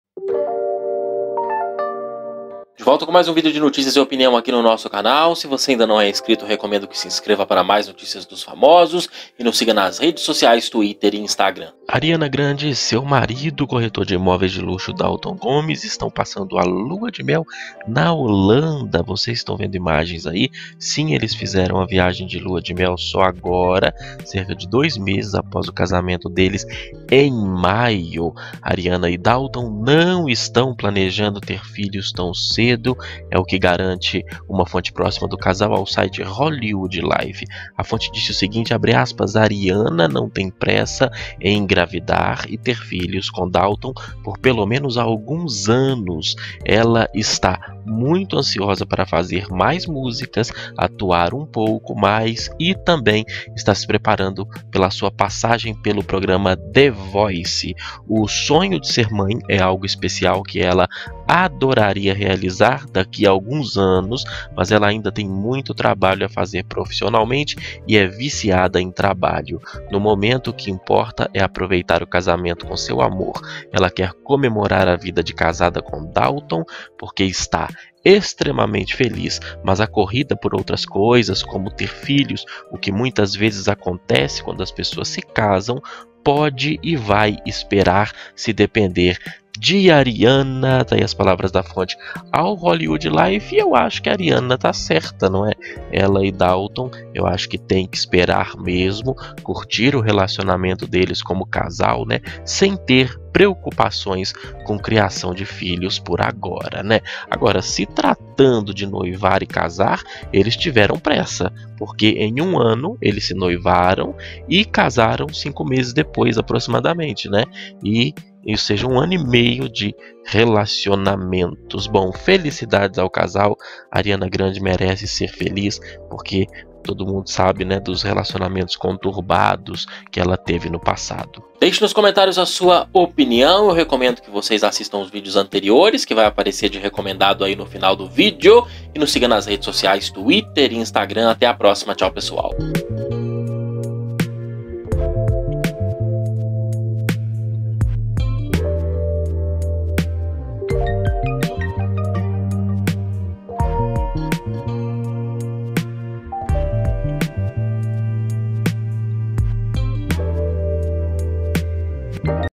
ARIANA GRANDE E DALTON GOMEZ NÃO TEM PRESSA PARA TER FILHOS "VAI ESPERAR", DIZ FONTE
Ariana Grande e seu marido, o corretor de imóveis de luxo Dalton Gomez estão passando a lua de mel na Holanda, sim, eles fizeram a viagem de lua de mel só agora, cerca de 2 meses após o casamento deles, em maio.
Ariana e Dalton nao estão planejando ter filhos tão cedo, é o que disse uma fonte próxima do casal. Veja minha opinião sobre o assunto.